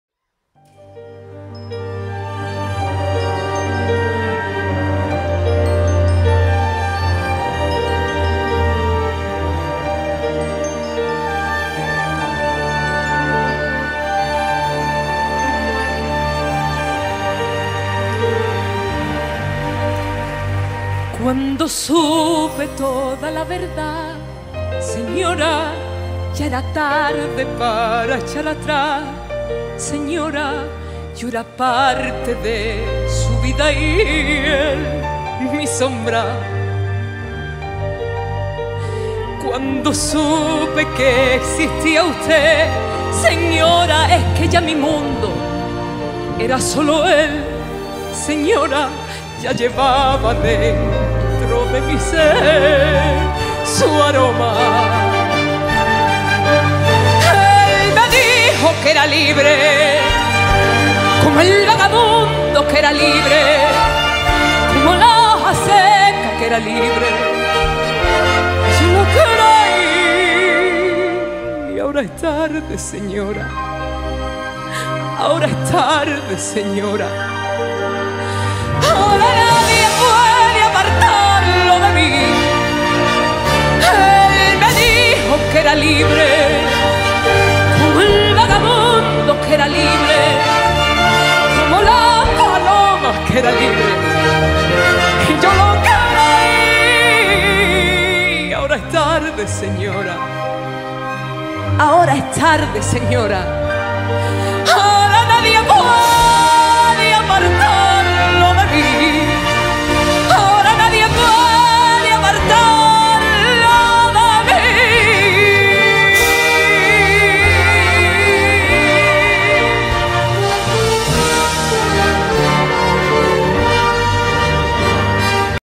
Laura Gallego resumen concierto Conil 27 de Agosto de 2021
Resumén del concierto de Laura Gallego en el Recinto La Chanca en Conil de la fra. el dia 27 de Agosto de 2021.
Se agotaron las entradas en 48 horas.